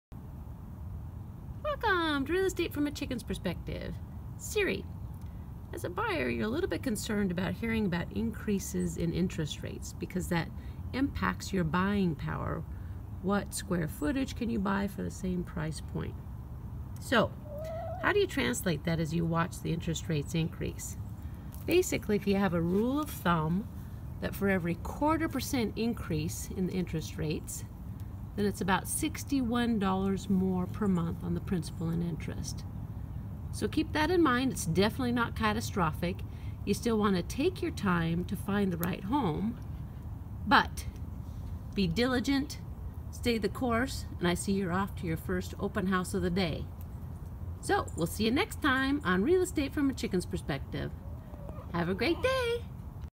Real Estate from a Chickens Perspective
With rising interest rates, how much more per month will that be in a house payment when buying a home?